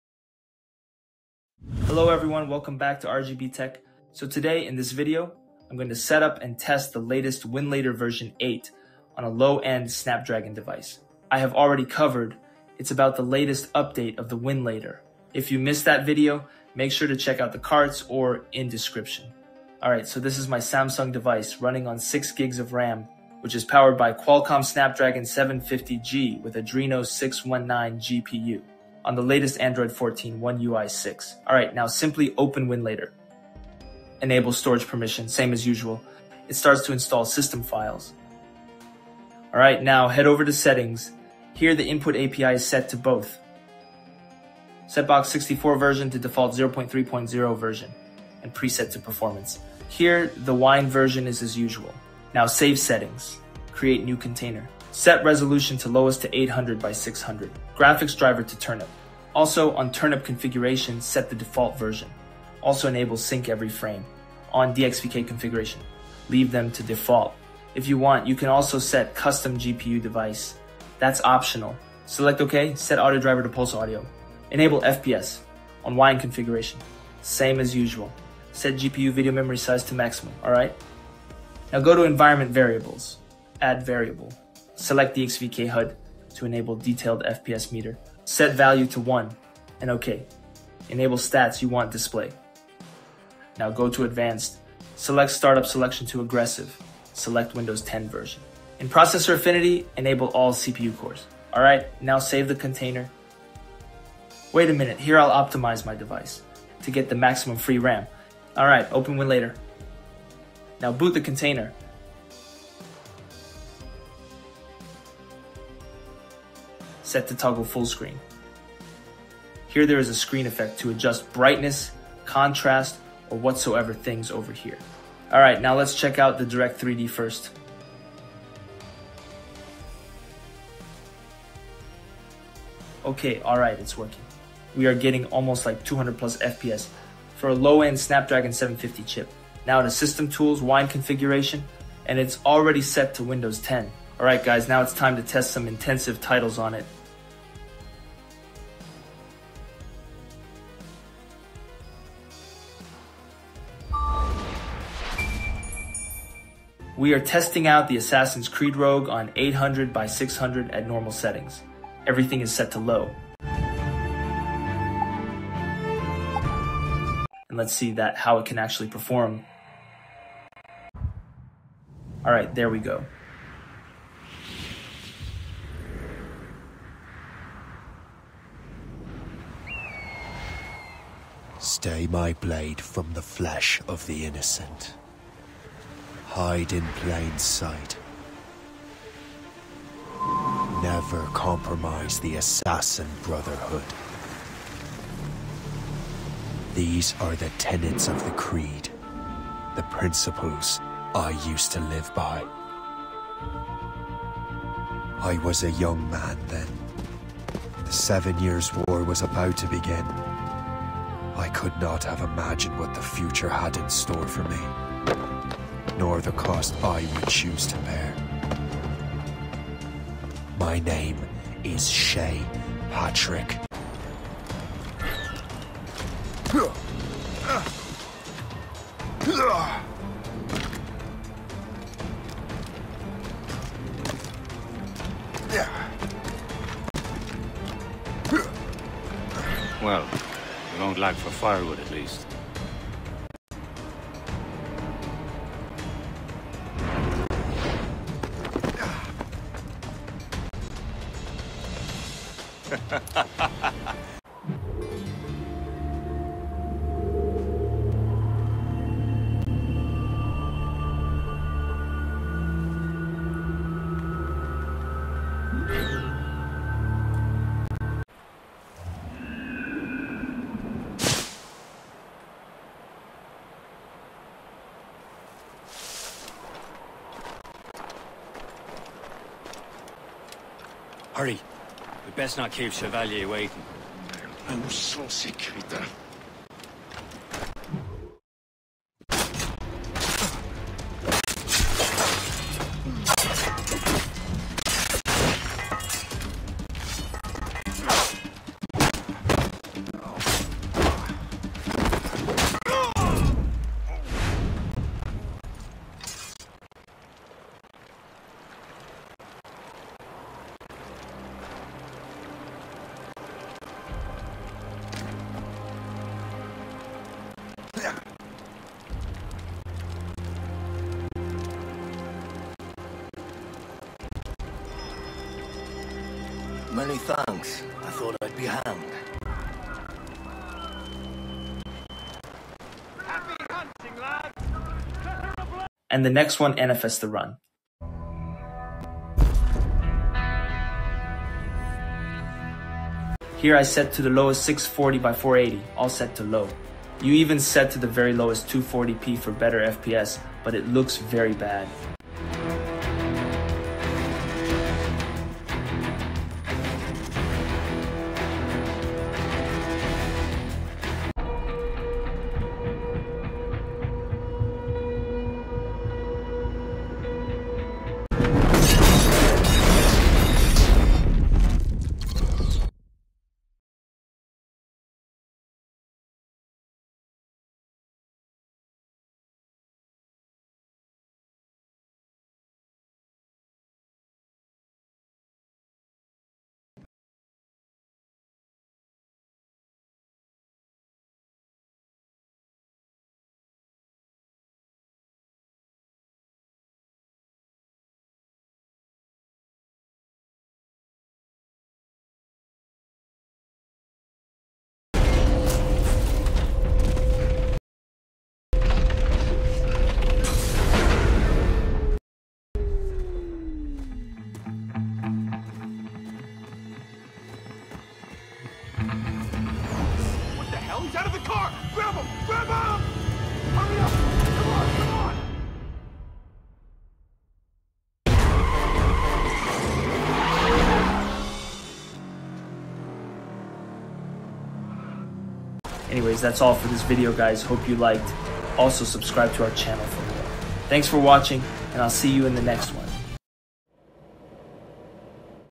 WINLATOR On Low-End Android Phone Test - Setup & Best Settings!
0:00 winlator setup
2:25 Game Test 1
5:35 Game Test 2

Phone Specifications:
➤ Model: Samsung Galaxy F23
➤ System OS: Android 14, One UI 6.1
EMU OS: Winlator v8, Wine 9.2
➤ CPU: Qualcomm Snapdragon 750G (8nm), ARMv8.3-A, 5W TDP
Cores: 2.2 GHz x2 Kyro 570 (A77) + 1.8 GHz x6 Kyro 570 (A55)
➤ GPU: Adreno 619
GPU Cores: 256 ALUs, Upto 825Mhz
GPU FP32: 0.432 TFlops
GPU Features: DirectX 12.1, Vulkan 1.1, CL 2.0
➤ Display: FHD+ 2408x1080 LCD, HDR10, 120Hz
➤ 4500 Mah battery, 25W FC

Cheers!